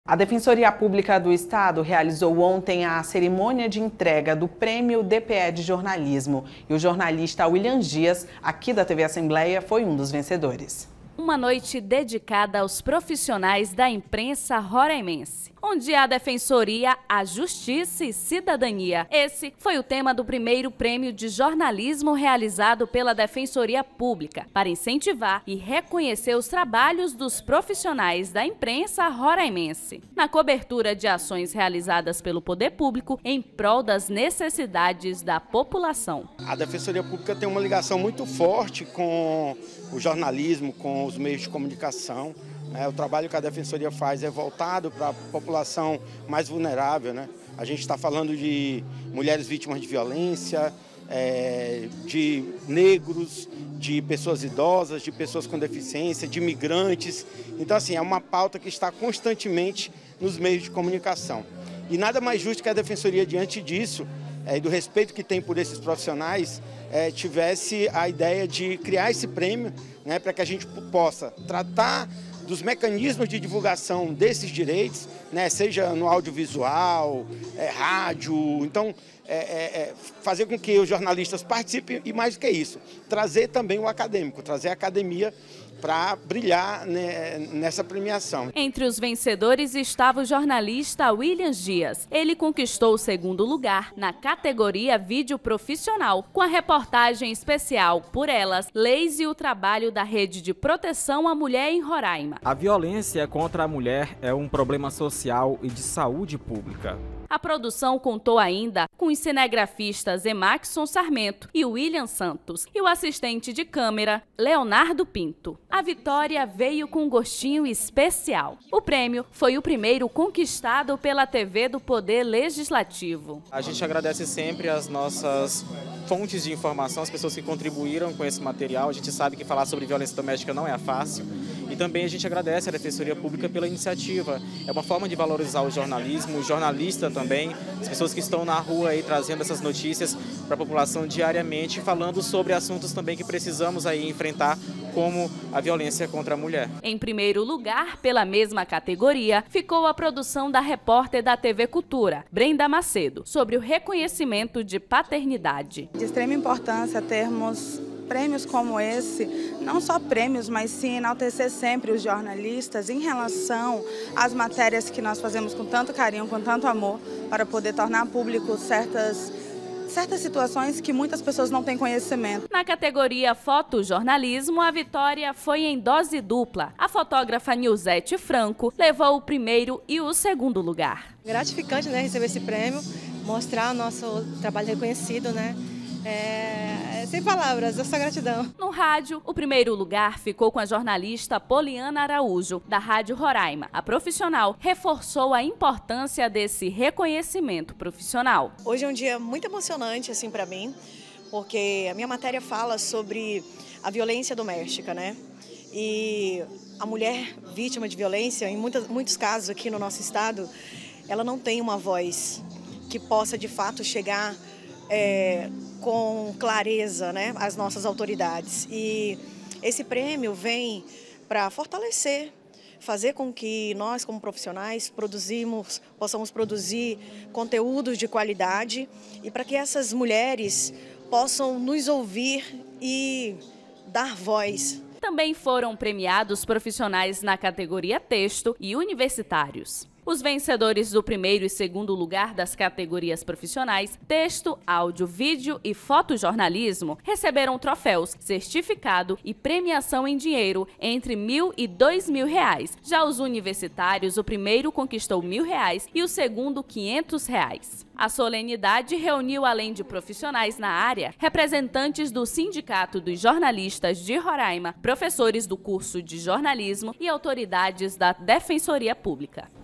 JORNALISTA DA ALE-RR GANHA PRÊMIO DPE DE JORNALISMO
A Defensoria Pública do Estado realizou a cerimônia de entrega do “Prêmio DPE de Jornalismo”. O jornalista Williams Dias, que integra a equipe da TV Assembleia, foi um dos vencedores.